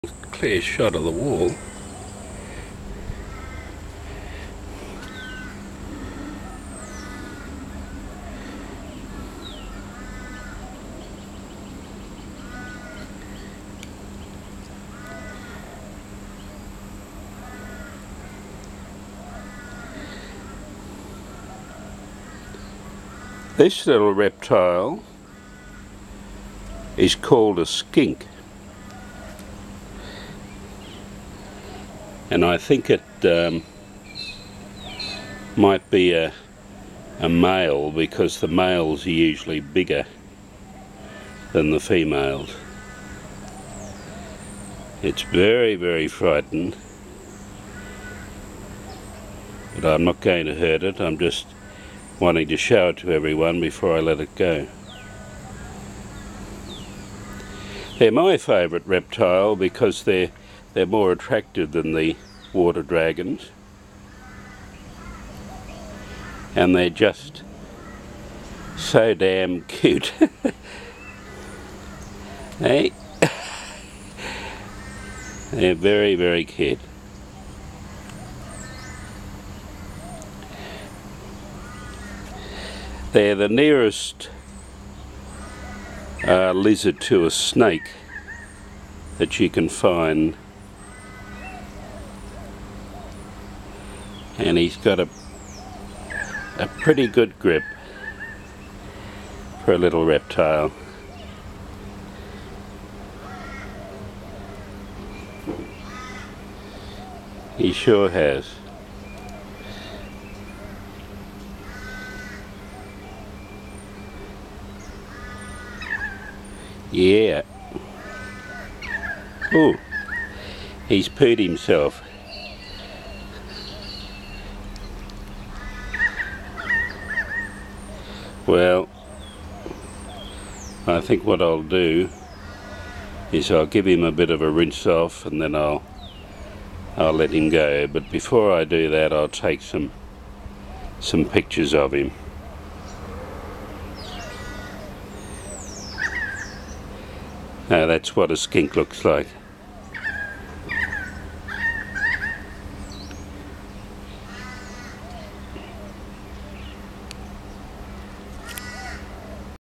A closer look at a Skink which is a type of small Lizard.
I found this Skink (I assume it's Male due to size) on the front Verandah and was able to Film it.  Unfortunately, the Subject is slightly out of Focus, but all Body parts can still be seen.